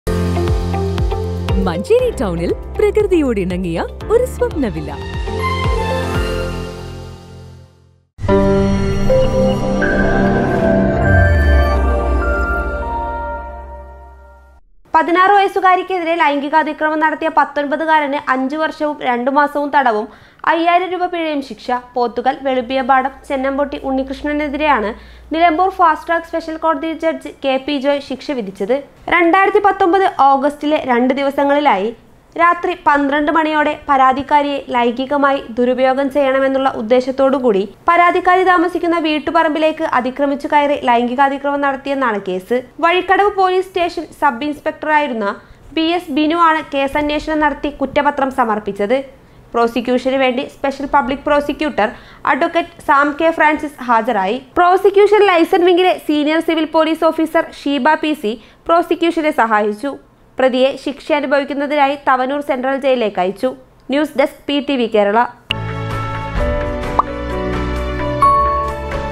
16 വയസ്സുകാരിക്കെതിരെ ലൈംഗികാതിക്രമം നടത്തിയ 19 കാരന് തടവും പിഴയും
16 വയസ്സുകാരിക്കെതിരെ ലൈംഗികാതിക്രമം നടത്തിയ 19 കാരന് തടവും പിഴയും